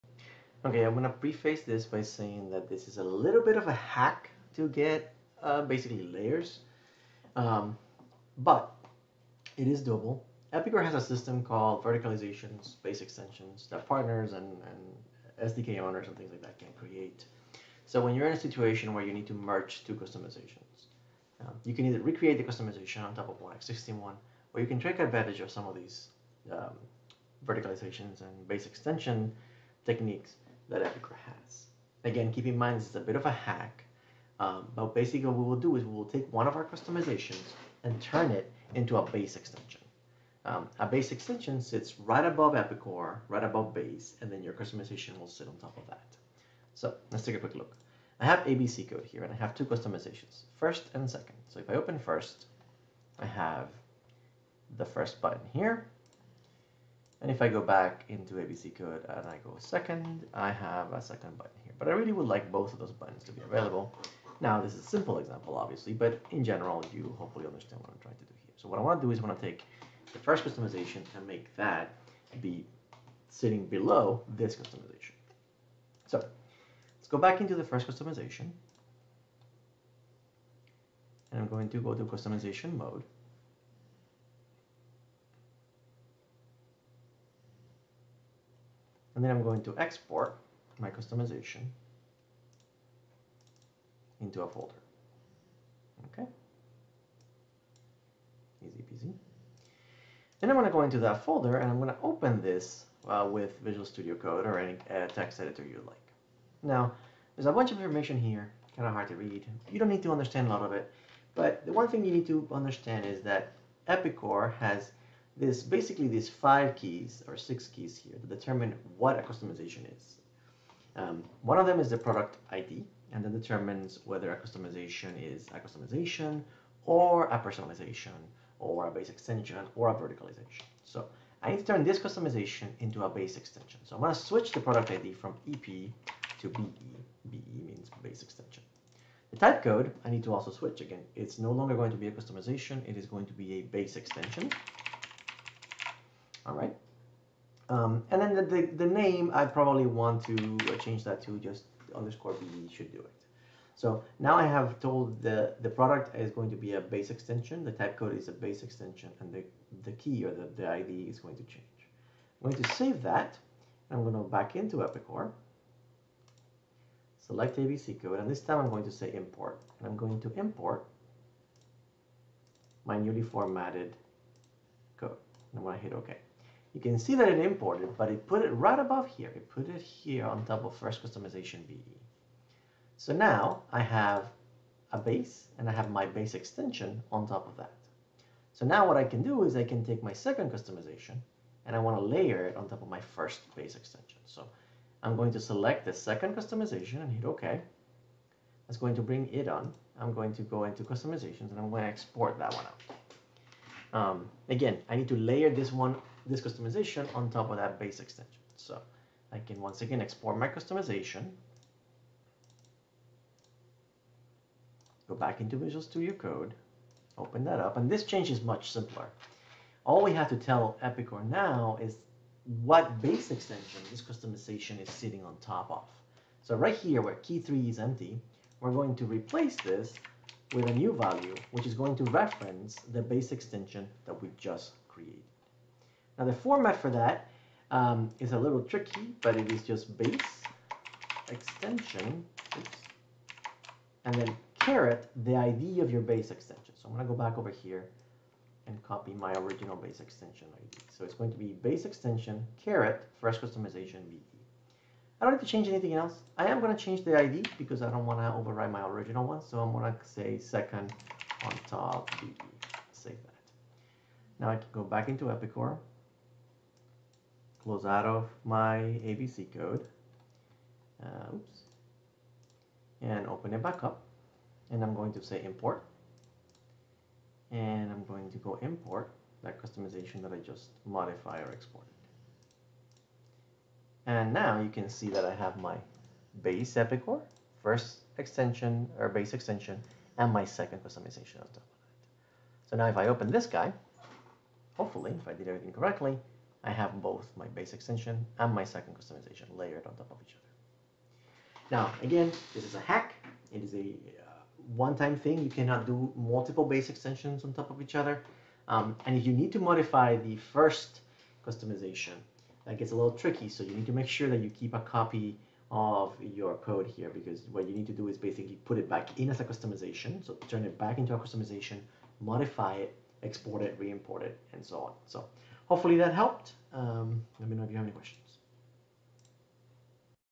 Epicor / Kinetic How to Create a Base Extension from a Customization to allow Layering.
This is a brief demo of how to accomplish the request on this thread. The question basically asks if there is a way to layer classic customizations. This video walks through how to accomplish this using Base Extension.